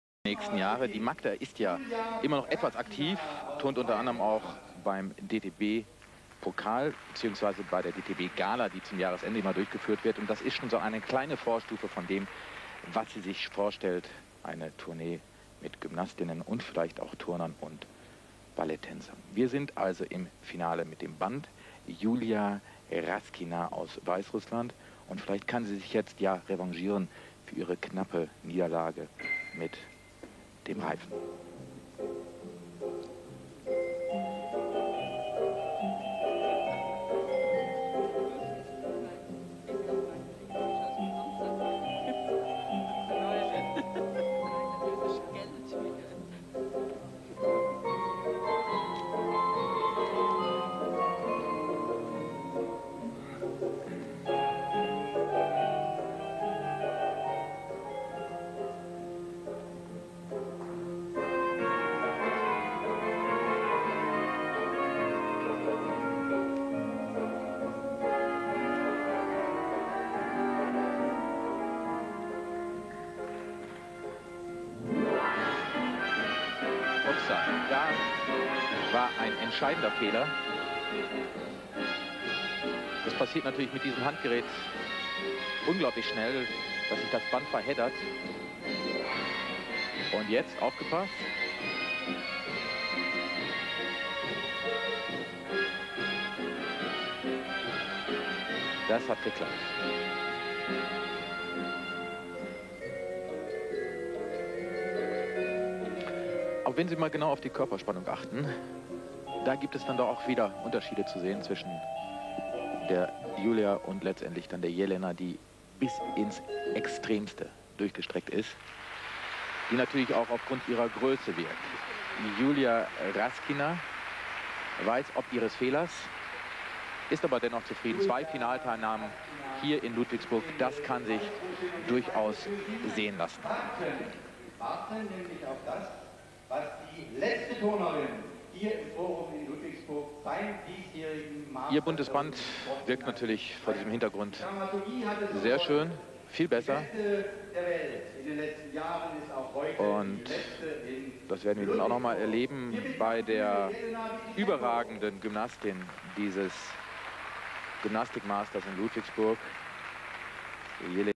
【新体操】ユリア・ラスキナ 1999年マスターズ杯 リボン
ユリア・ラスキナ（Yulia Raskina、Юлия Раскина、1982年4月9日 - ）はベラルーシの新体操選手。
1982年4月9日、ユダヤ系のスポーツ一家のもとに生まれる。母親は旧ソ連の新体操のコーチ。ジュニア時代は母親の指導を受けていた。
1989年より新体操を始める。
1992年にナショナルチーム入り。
1997年の世界新体操選手権に団体総合のメンバーとして出場し、銀メダル獲得に貢献した。
1999年の世界新体操選手権において、個人総合含む全4種目で銀メダルを獲得。
2000年のシドニーオリンピックの個人総合において銀メダルを獲得。1位のロシアの旗ユリア・バルスコワとは0.084点の僅差だった。
2003年に現役引退。